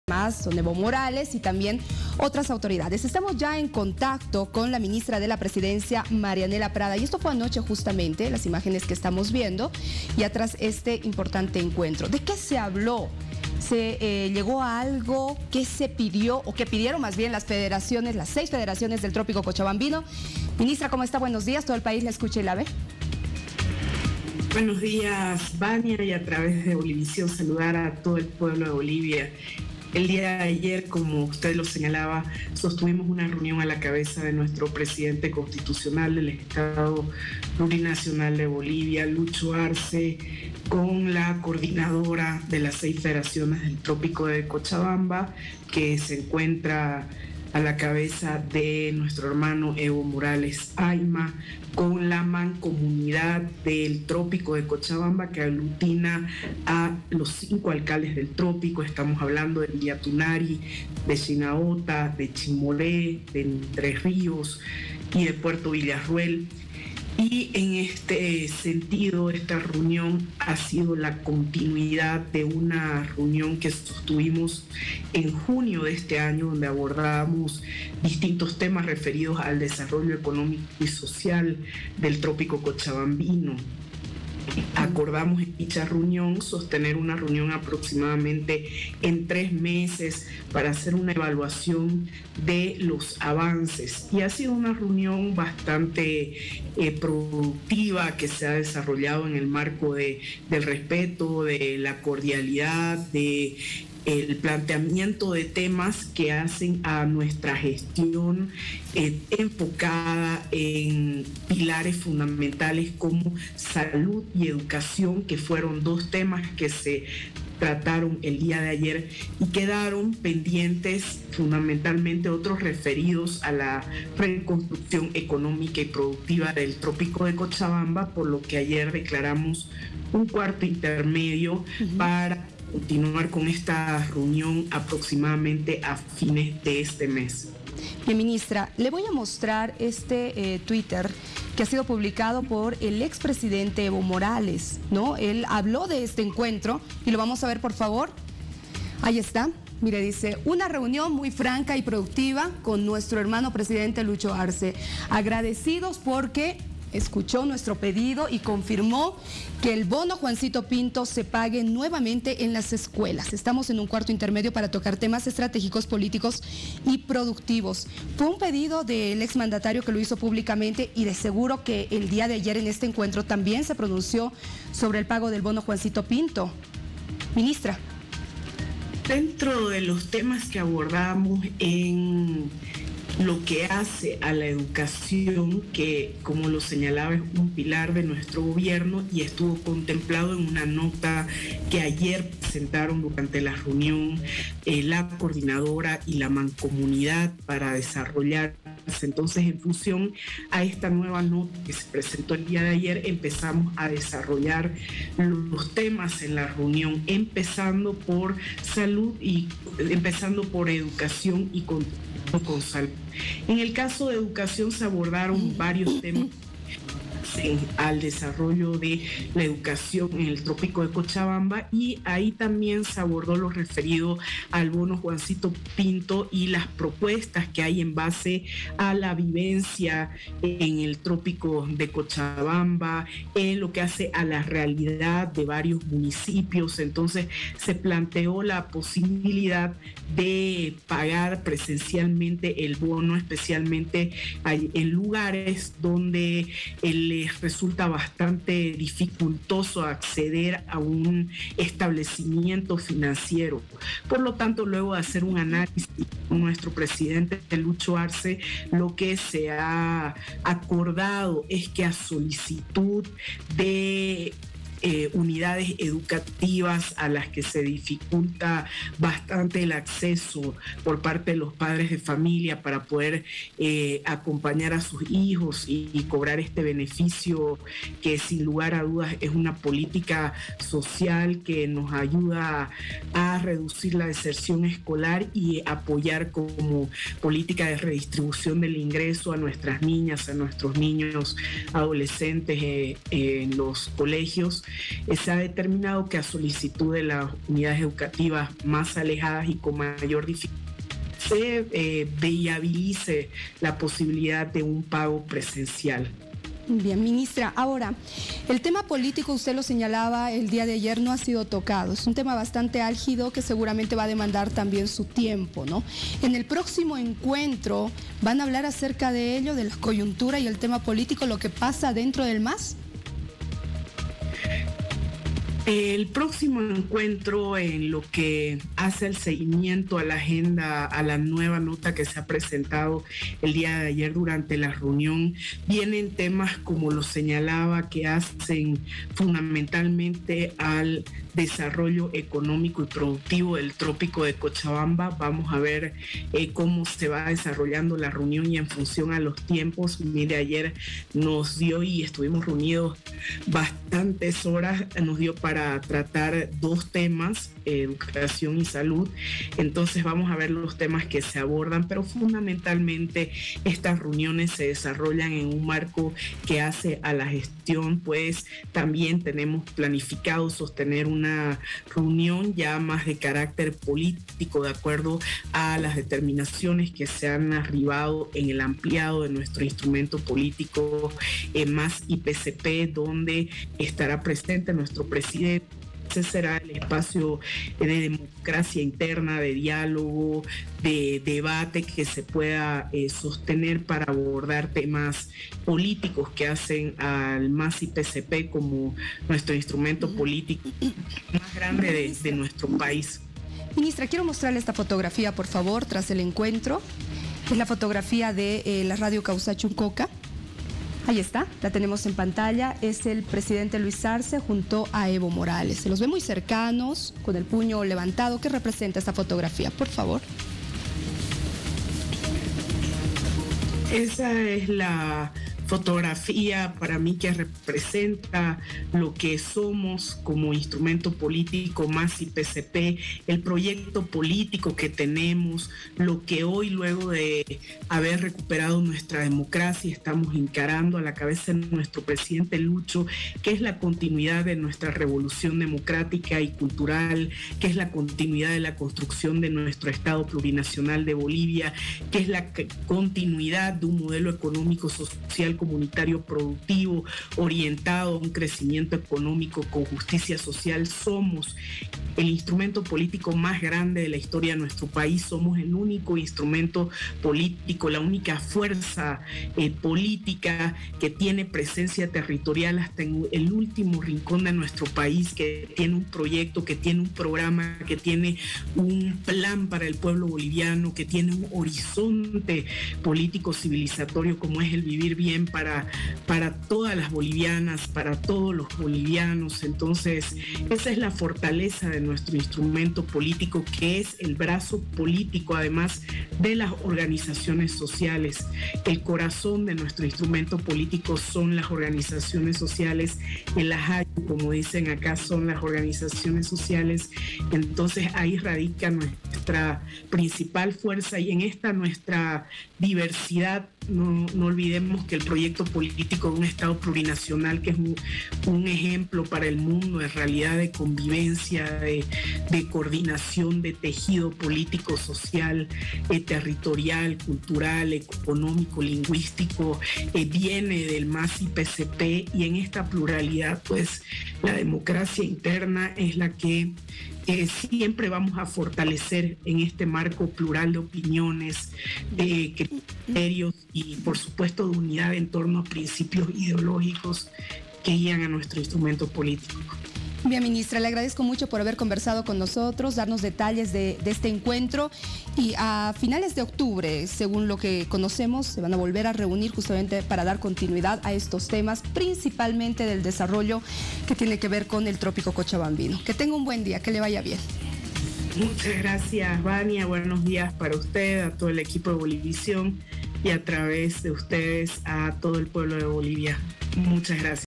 En Exclusiva con María Nela Prada
El presidente Luis Arce se reunió con 6 federaciones.
La Ministra de la Presidencia señala que la reunión donde participó el Presidente y representantes de 6 federaciones, se tuvo como temas bases salud y educación.